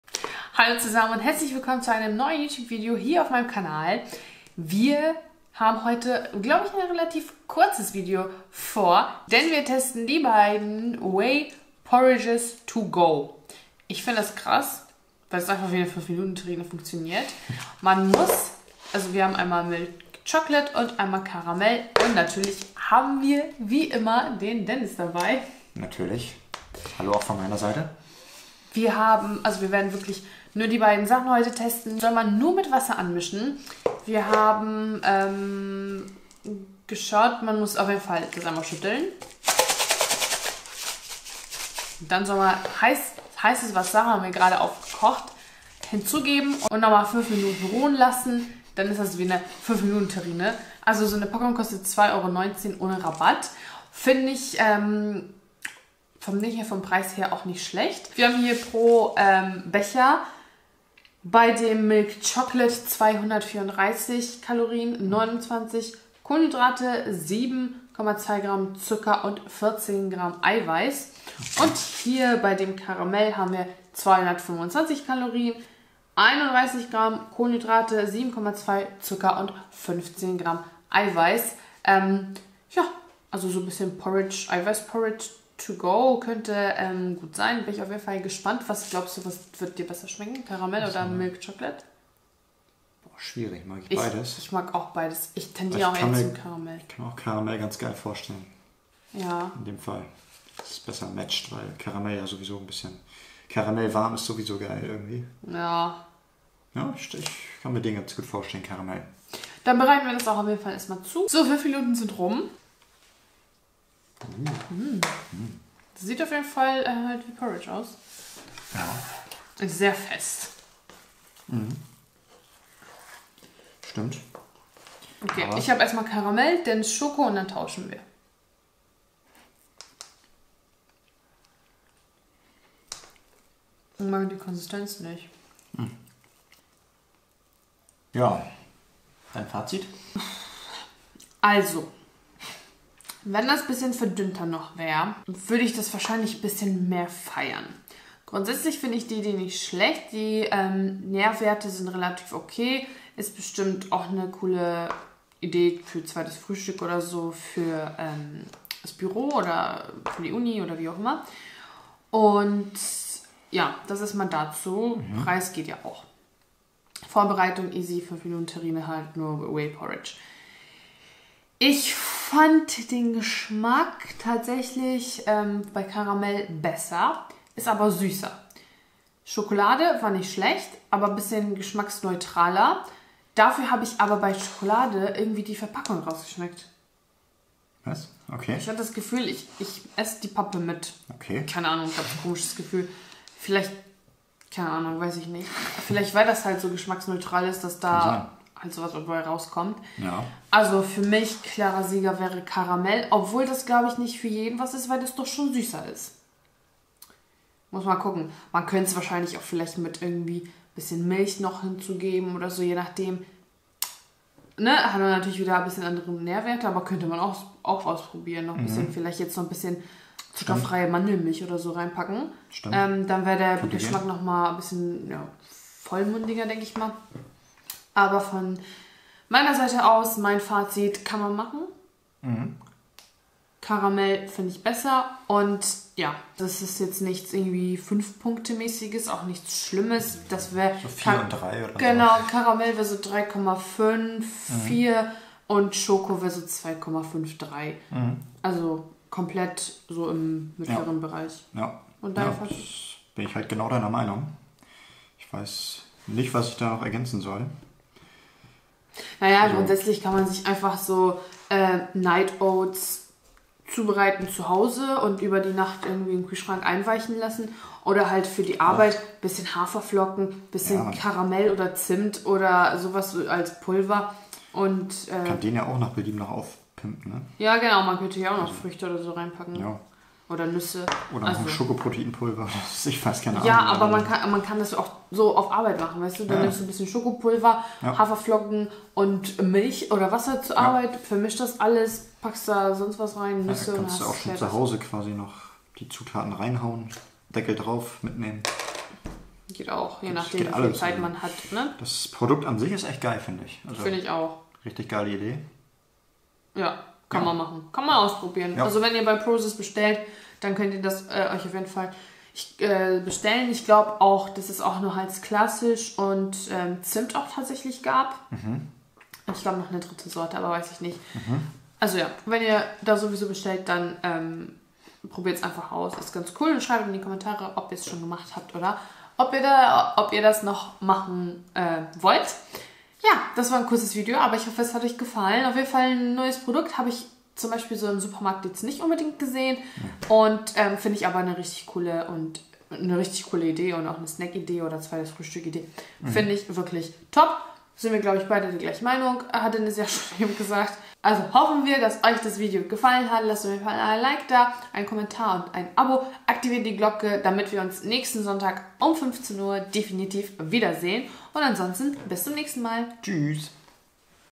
WIR TESTEN WHEY PORRIDGE TO GO - PROZIS I SCHOKOLADE I KARAMELL
Wir testen WHEY PORRDIGE TO GO von Prozis für euch!

Anzeige/Markennennungen - keine Kooperation

Folgende Produkte haben wir getestet:

1. Whey Porridge To Go - Schokolade
2. Whey Porridge To Go - Karamell

Wir haben den Geschmack bewertet und verraten euch unseren Favoriten. Im Video haben wir auch von jeder Sorte die Nährwerte und den Preis genannt.

Habt ihr es auch schon probiert? Was haltet ihr von der Idee? ⬇️

Schreibt gerne in die Kommentare, welche Produkte wir noch für euch testen sollen!

Lasst ein Like, ein Kommentar und ein Abo da - um keine weiteren Videos zu verpassen. Jeden Sonntag kommt ein neues Tester-Video!